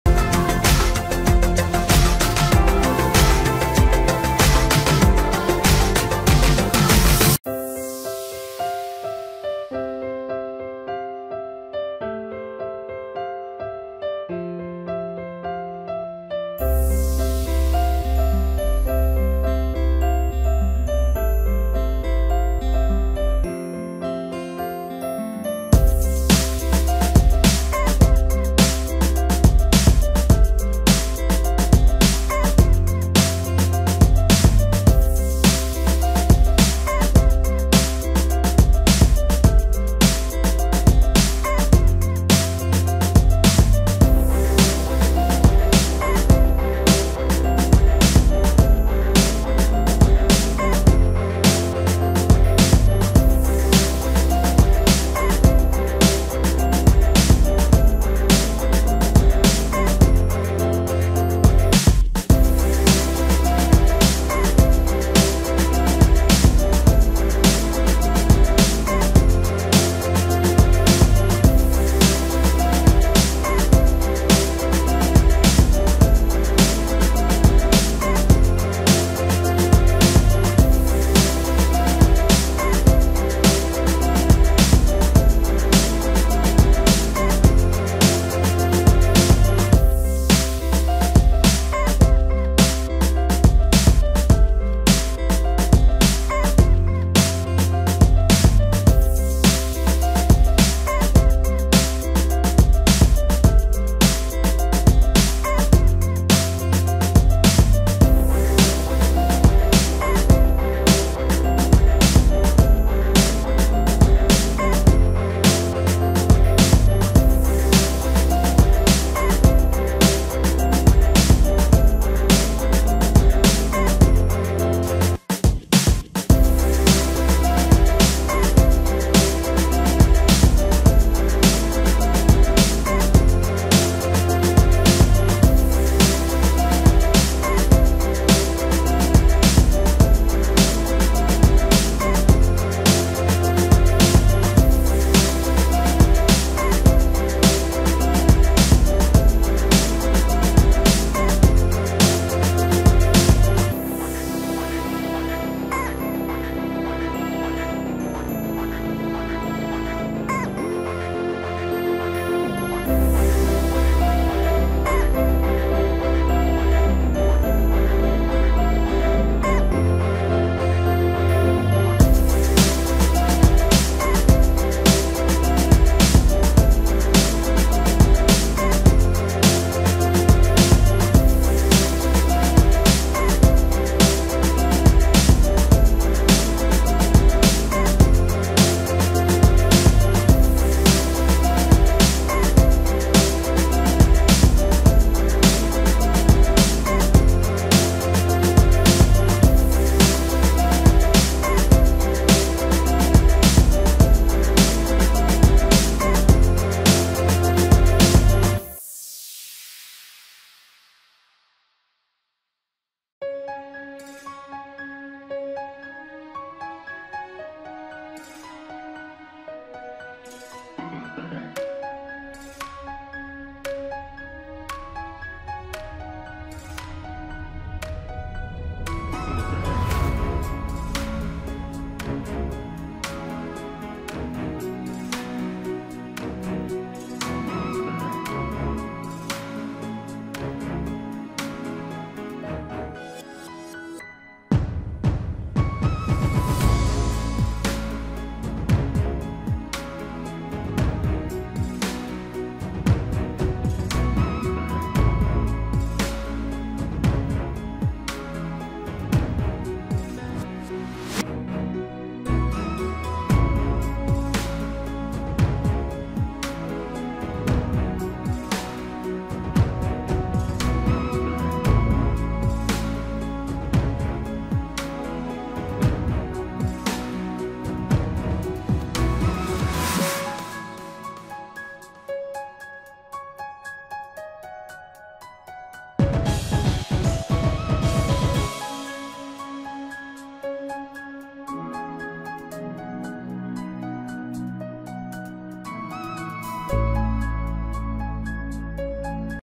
How to draw Anti Virus Sans Monster mode in the game Among Us
video I shot how to draw sans style in the game Among us in monster mode. how to draw is very beautiful and lovely. Hope to bring you a creative experience.